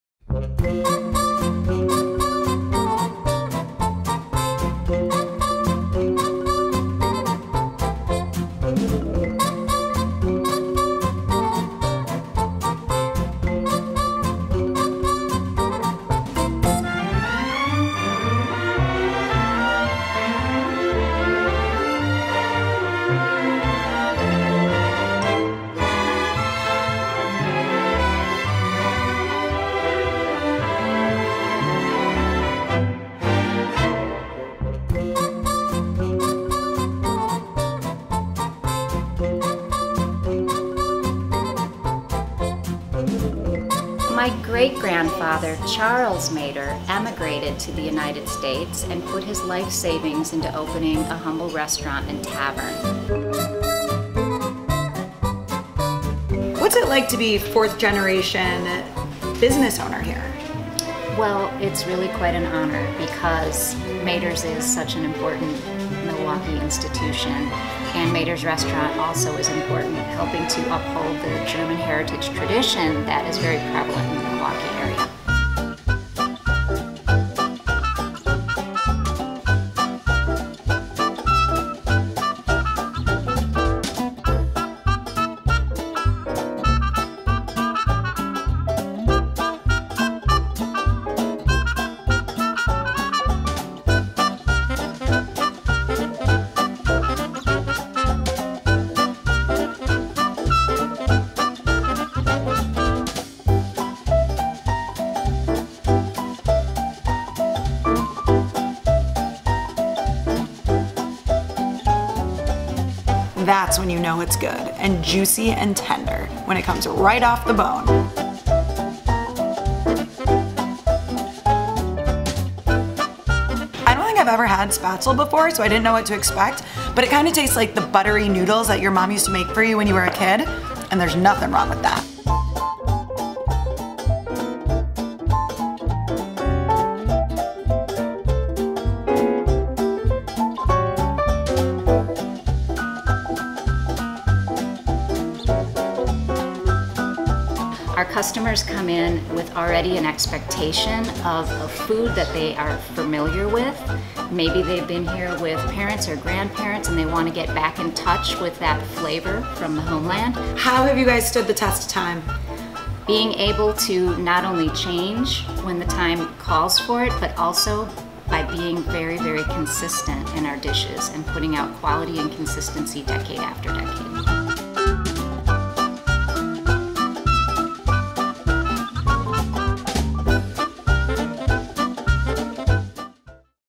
Traditional German Restaurant In Milwaukee Serves Giant Pretzel
Mader’s restaurant has been serving traditional German food since 1902. The oldest restaurant in Milwaukee, Wisconsin, it has been family-run for four generations. Everything from the food and drinks to decor and costumed waitstaff celebrate German heritage.